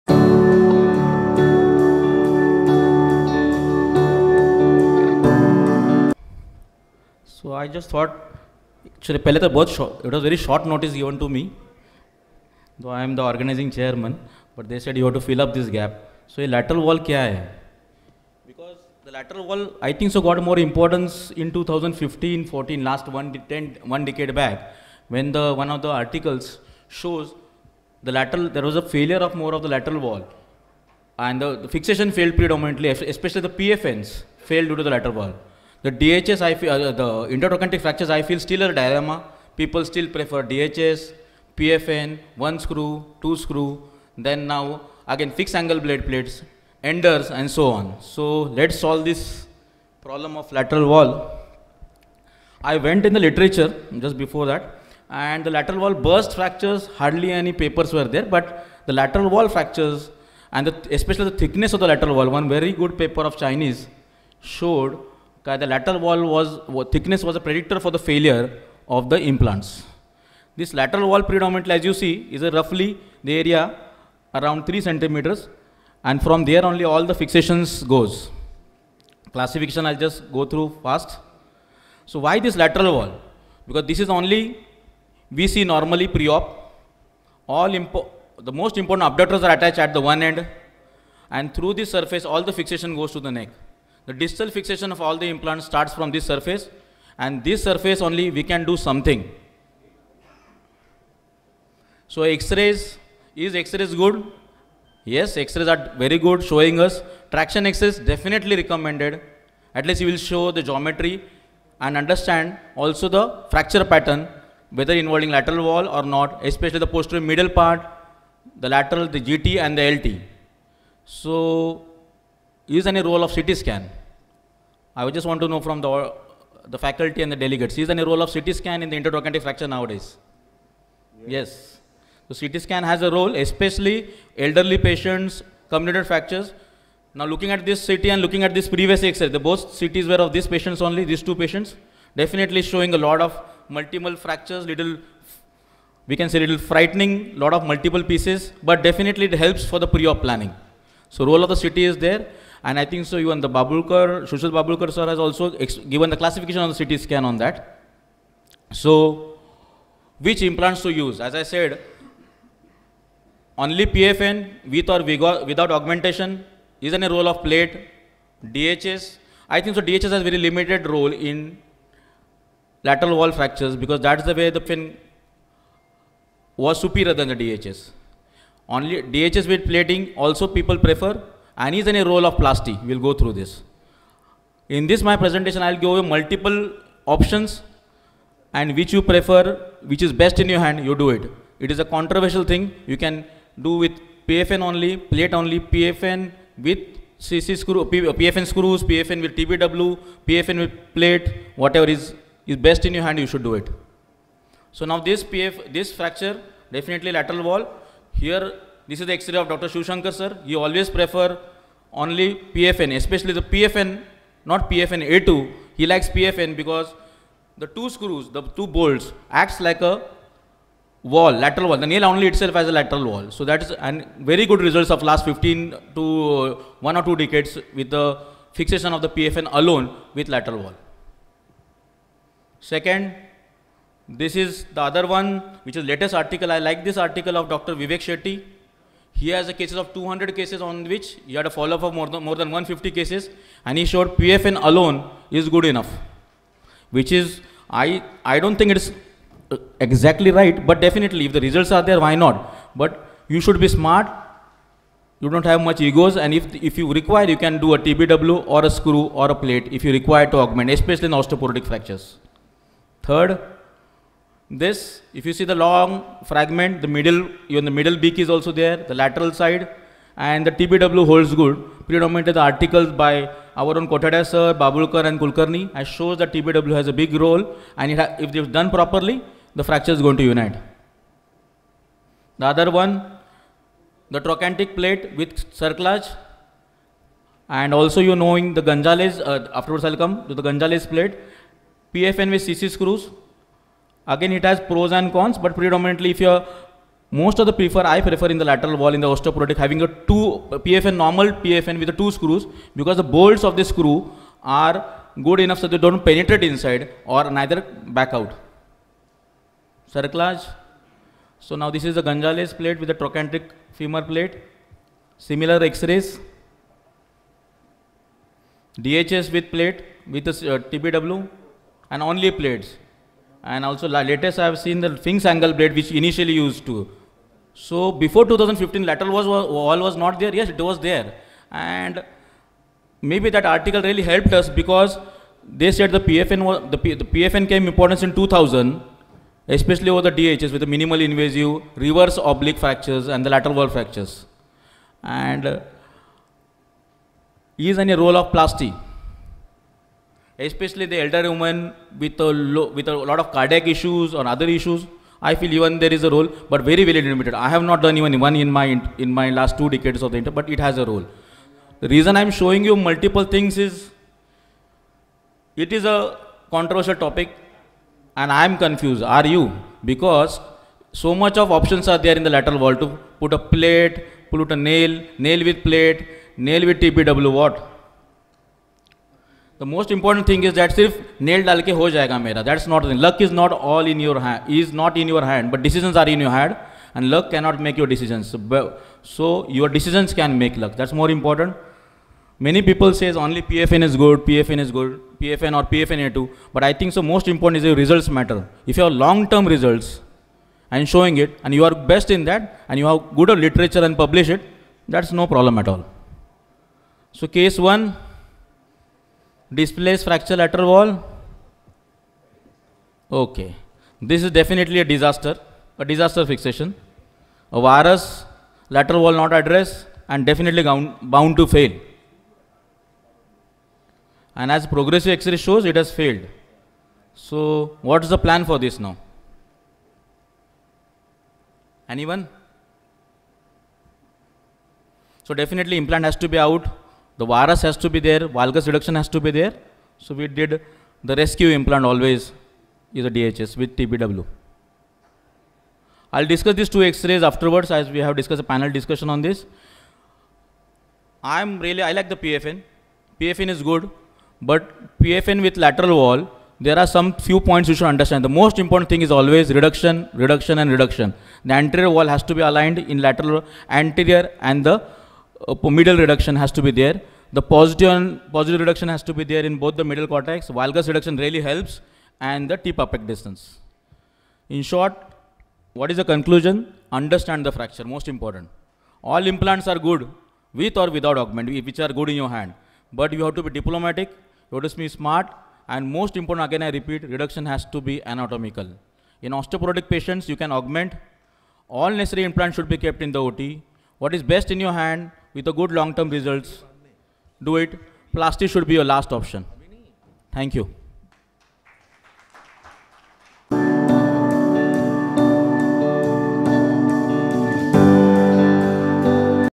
NMOACON 2024 : Intertrochantric Fractures with Lateral Wall Burst - Dr. Sachin Kale
This talk is part of Navi Mumbai Orthopaedic Association

Venue - Marriott Executive Apartments, Navi Mumbai

Organising Chairman - Dr. Sachin Kale
Organising Secretary - Dr Pramod Bhor
Scientific Committee Chairman - Dr Sanjay Dhar